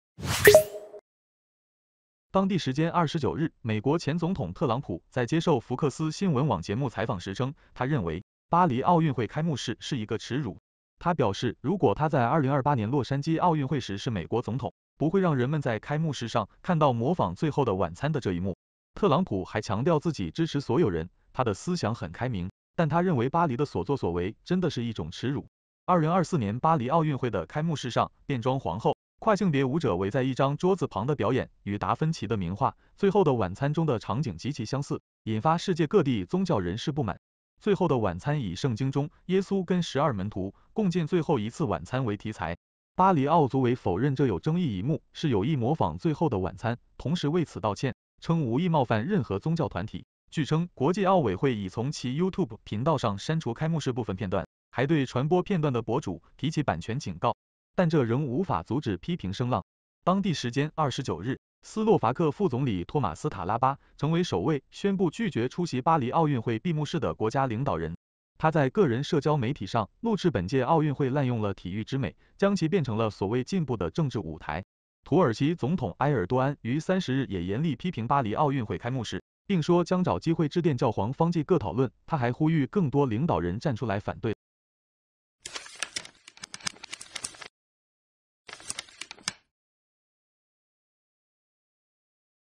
特朗普称：巴黎奥运会开幕式是“耻辱”
当地时间29日，美国前总统特朗普在接受福克斯新闻网节目采访时称，巴黎奥运会开幕式“是一个耻辱”。